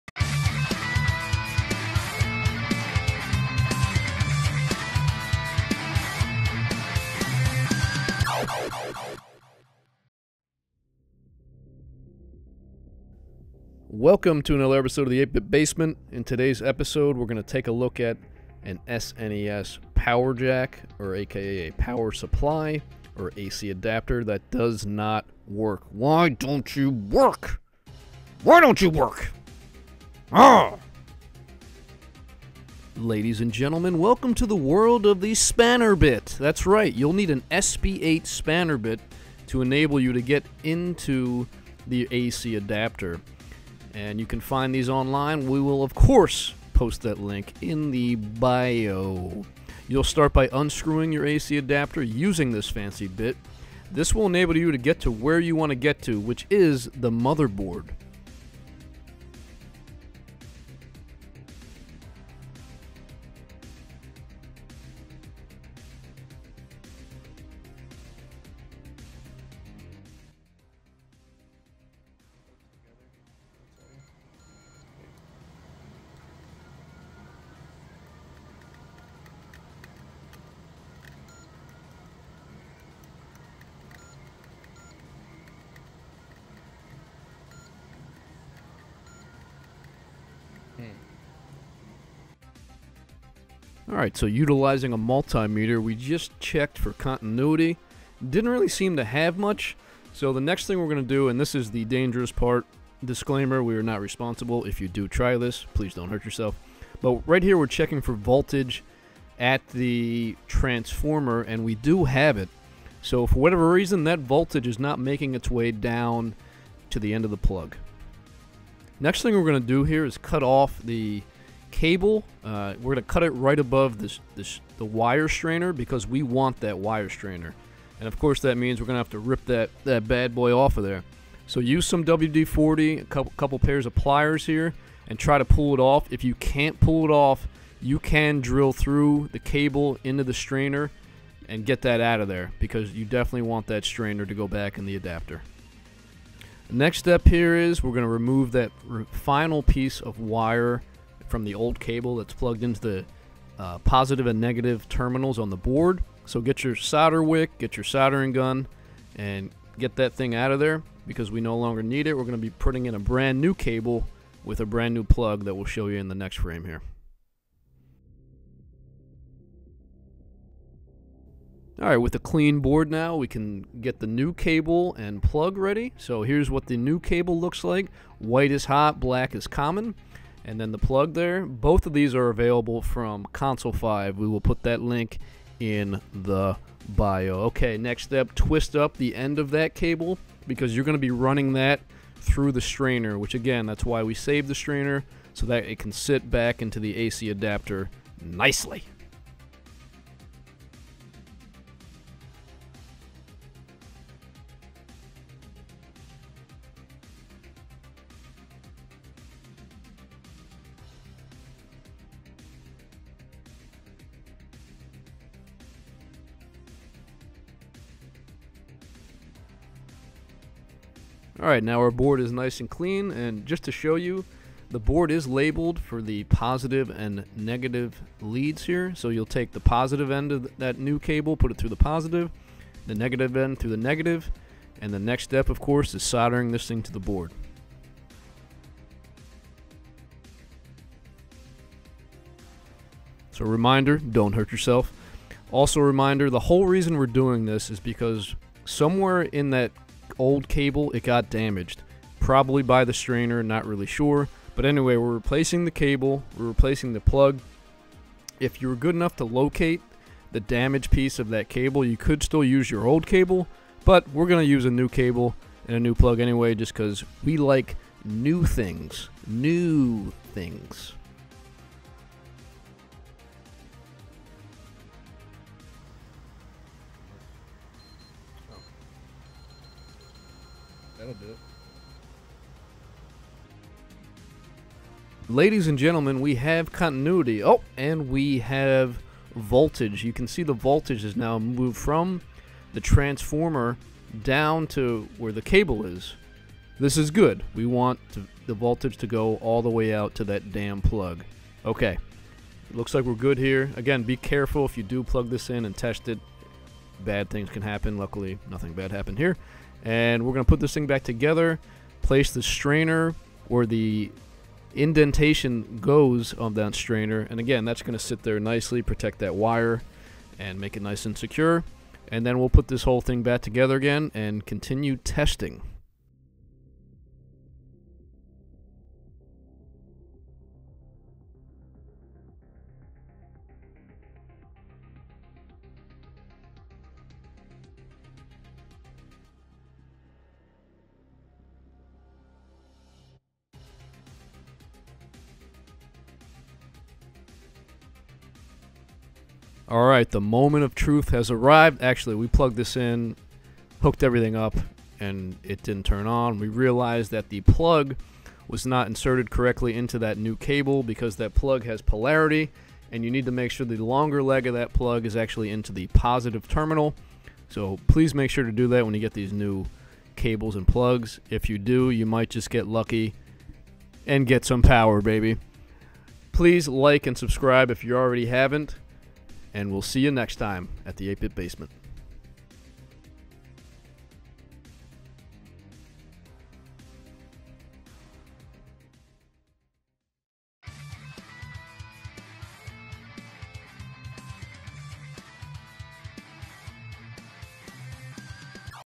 How To Fix Your Broken SNES AC Adapter! | SNES AC Adapter Repair
If you've identified your SNES AC Adapter as being no good, do NOT trash it. FIX IT! This video shows how to get that adapter back in shape with a brand new wire and plug from Console5! See below for some other helpful repair vids to aid you.

Spanner SP8 Bit

👕Be A Walking Billboard for Us!